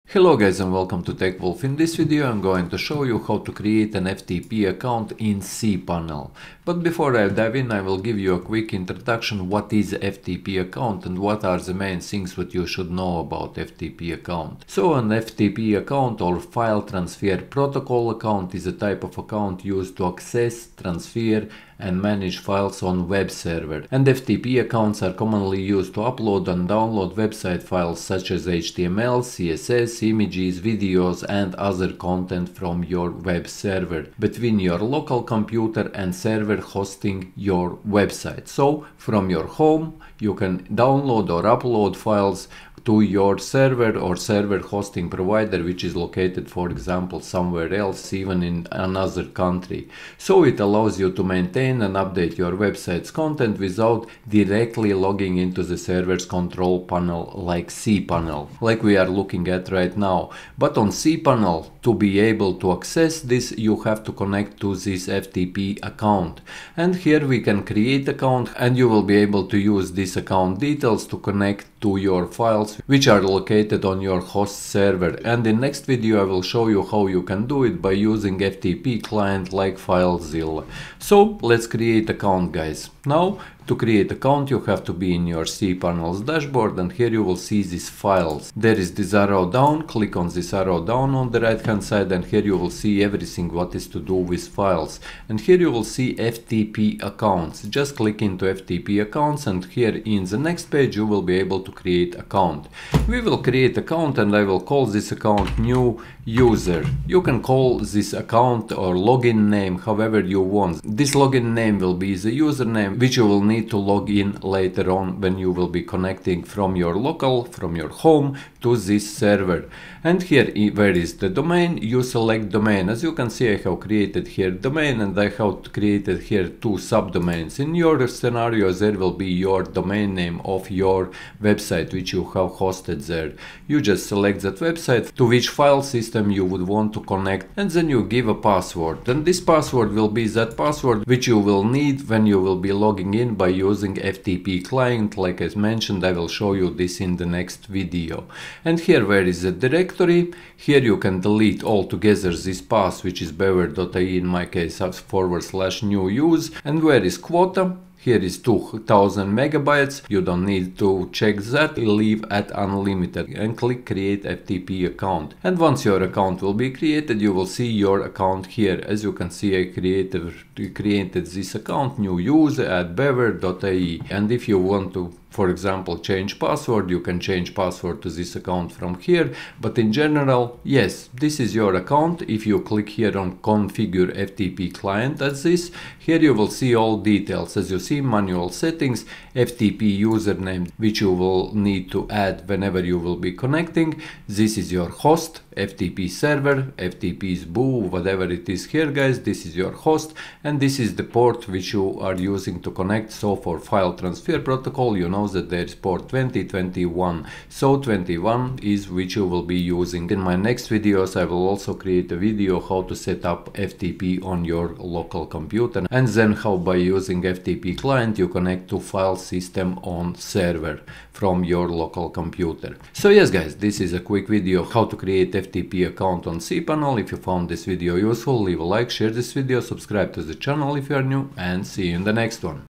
How To Create FTP Account In cPanel
Learn How To Create FTP Account In cPanel

An FTP (File Transfer Protocol) account is a type of account used to access, transfer, and manage files on a web server. FTP accounts are commonly used to upload and download website files, such as HTML, CSS, images, videos, and other content, between your local computer and the server hosting your website. It allows you to maintain and update your website's content without directly logging into the server's control panel, like cPanel.